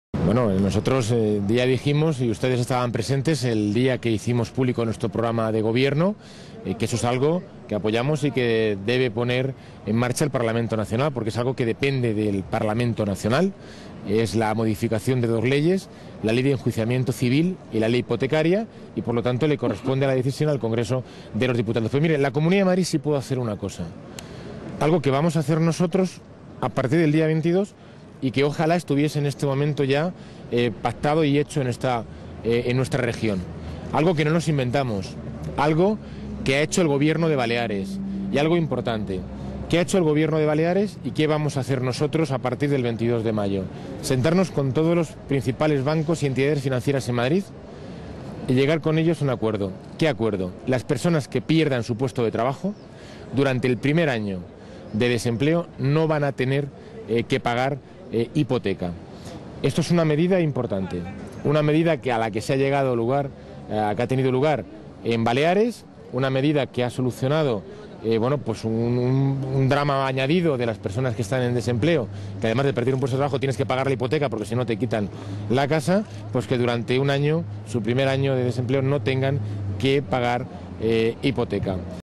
Gómez promete pacto con bancos para que parados no paguen hi
El secretario general del PSM y candidato del PSOE a la Presidencia de la Comunidad de Madrid, Tomás Gómez, ha estado de acuerdo con la propuesta hipotecaria que ha lanzado esta mañana Esperanza Aguirre, al tiempo que ha prometido un pacto con los bancos para que los desempleados no paguen hipoteca el primer año en el paro.